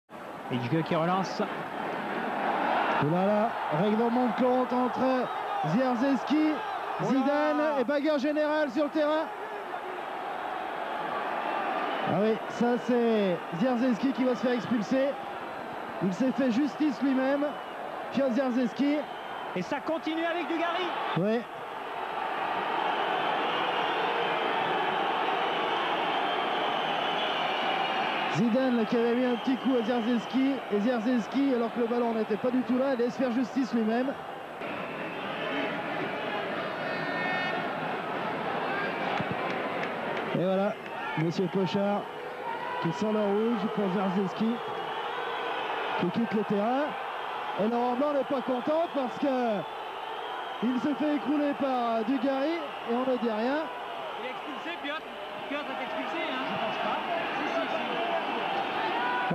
Asse bordeaux 1994-1995 bagarre générale tacle assassin
Bagarre générale pendant le match asse bordeaux durant la saison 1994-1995. Un match de dingue!!!